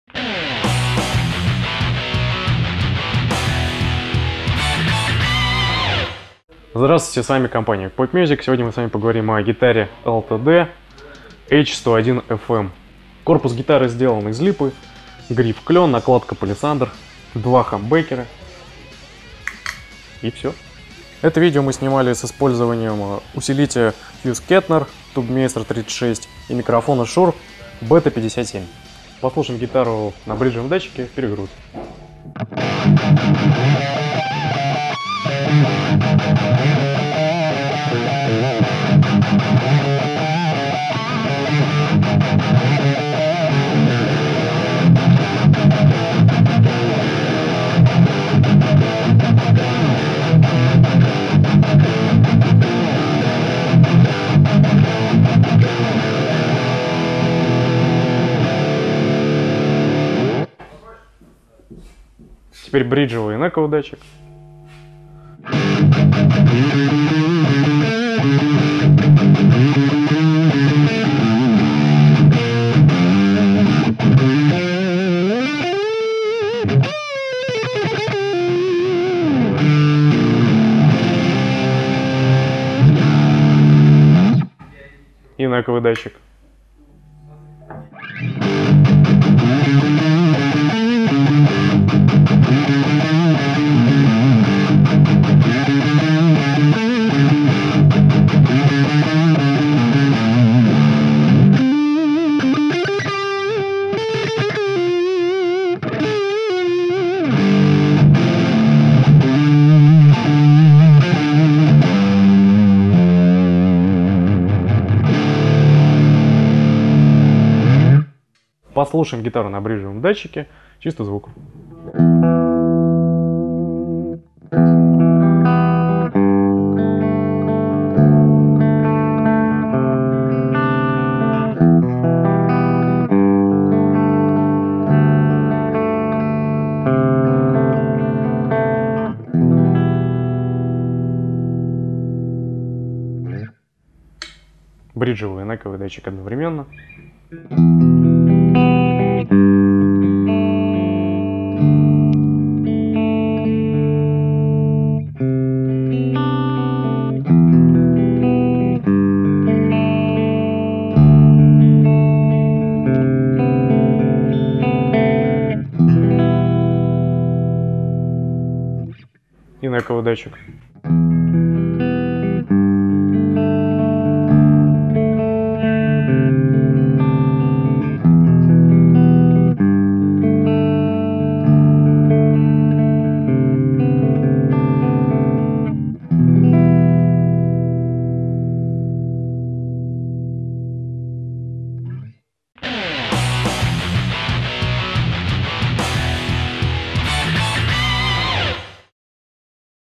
Электрогитара LTD H-101FM
0:07 Краткий рассказ о характеристиках электрогитары
0:32 Звучание гитары на перегрузе
2:02 Демонстрация звучания гитары на чистом звуке, все звукосниматели по очереди